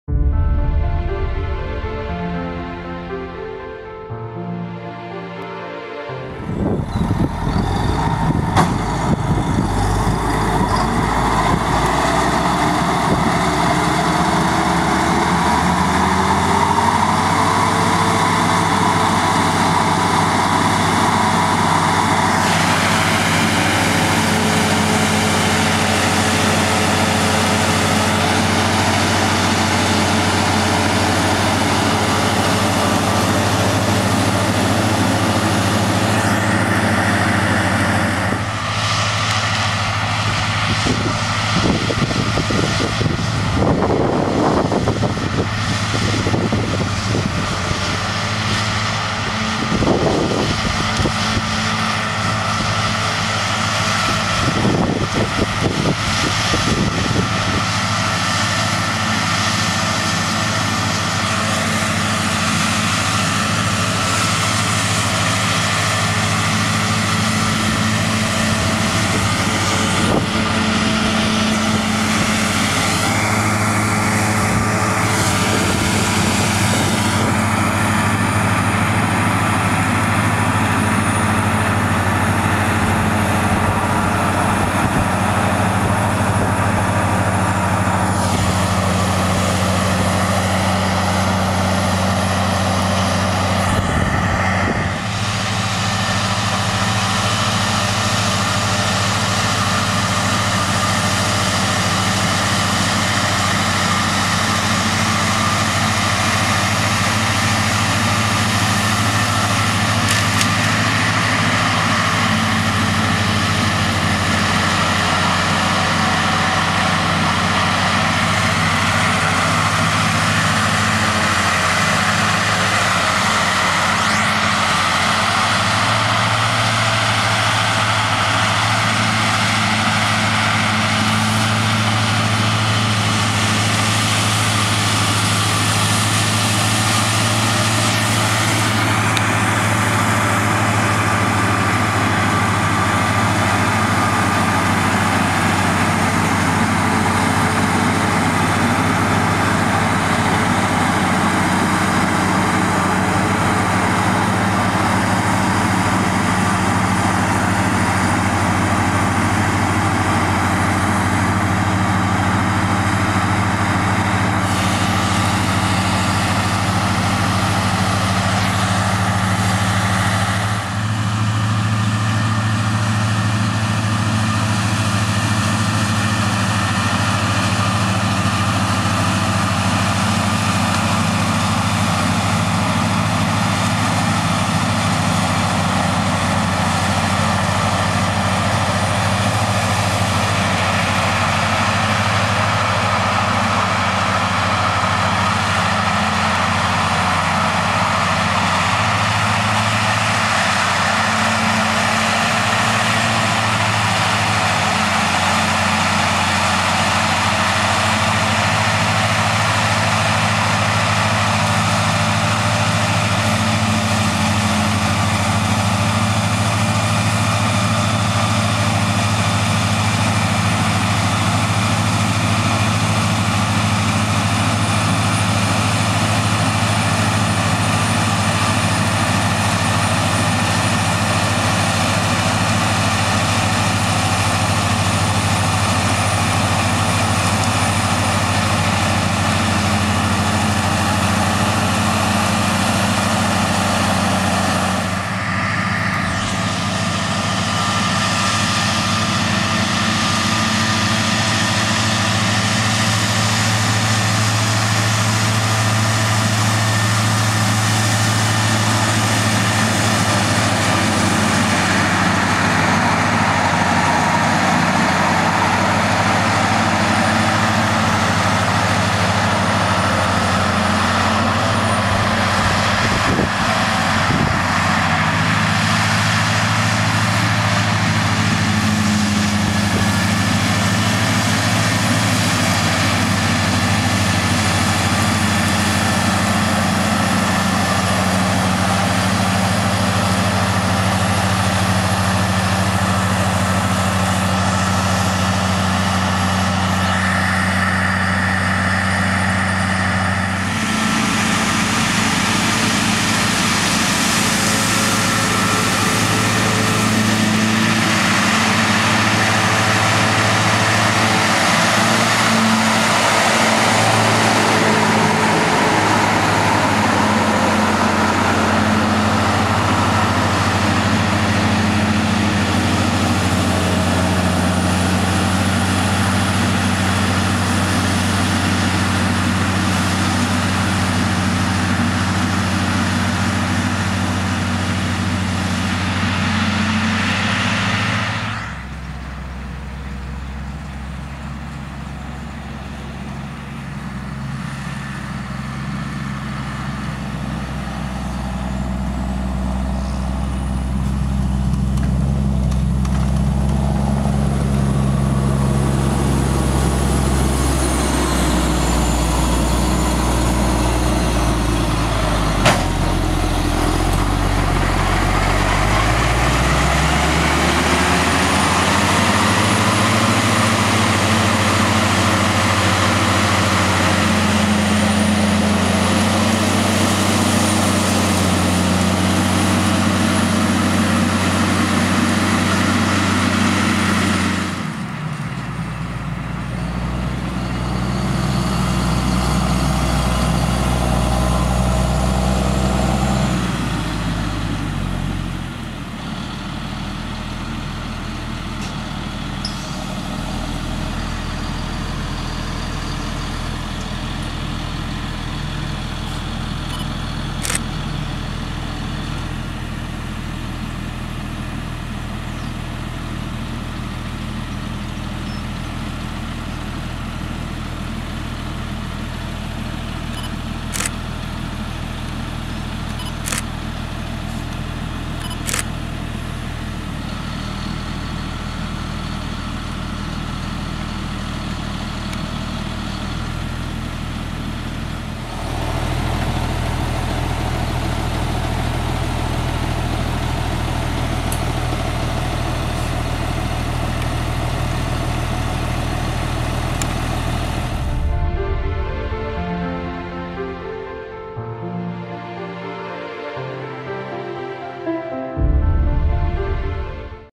Silage 2022 - Claas 3300 Xerion & Claas Triples Mowing (2/2)
Claas 3300 Xerion Reverse Drive & Claas Triples Mowing Grass Part 2/2.